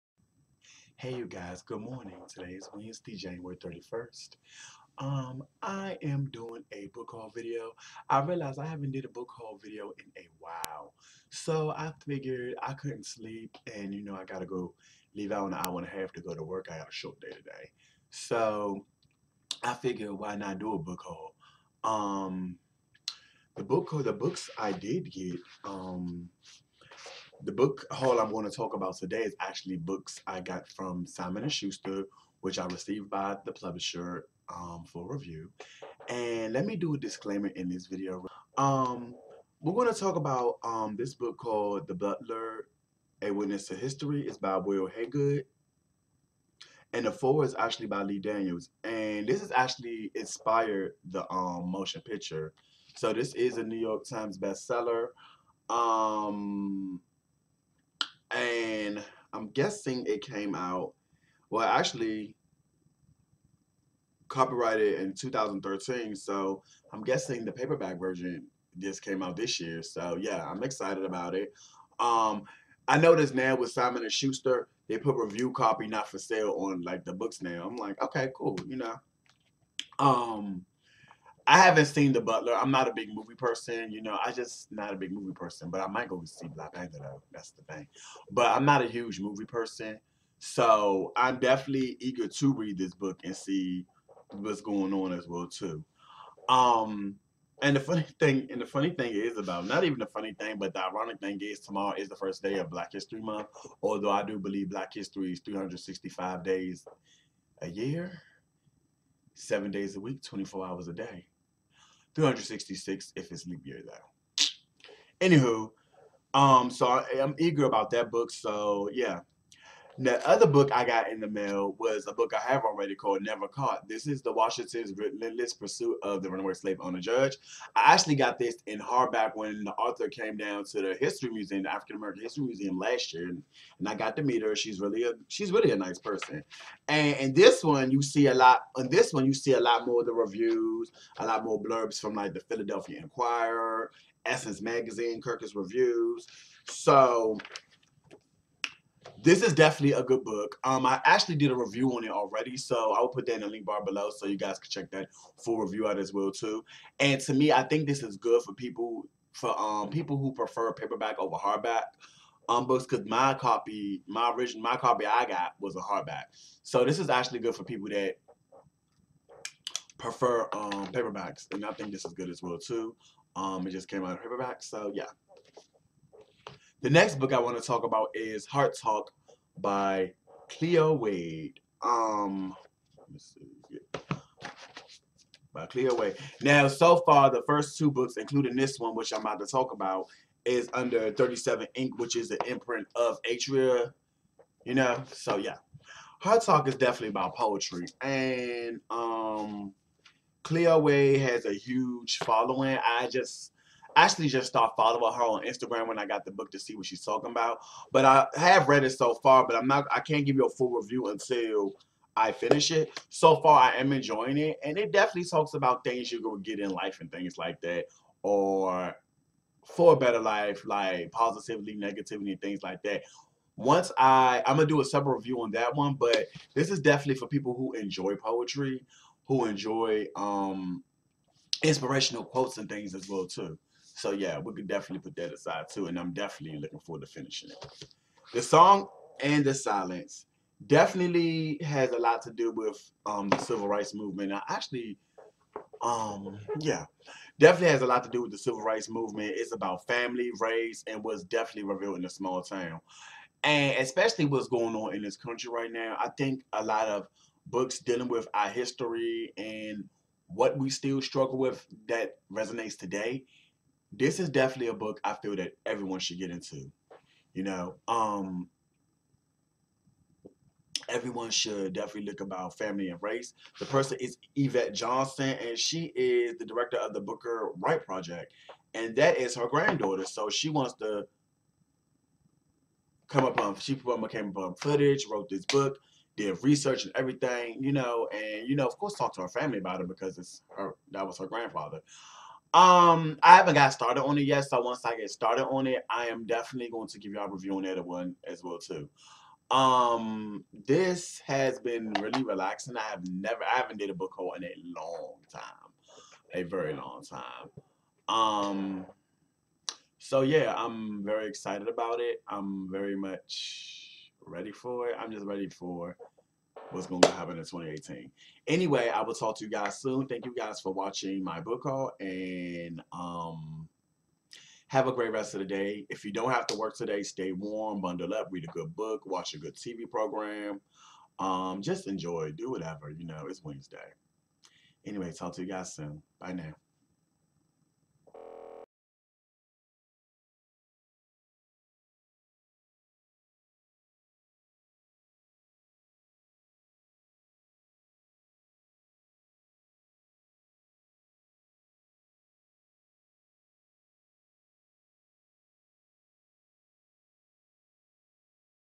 Book Haul: Simon and Schuster 37 Ink, Atria
My book haul were reviewer copies of books from Simon and Schuster. It includes: Heart Talk by Cleo Wade. The Song and the Silence by Yvette Johnson. Never Caught (paperback) by Erica Armstrong Dunbar and The Butler: A Witness To History.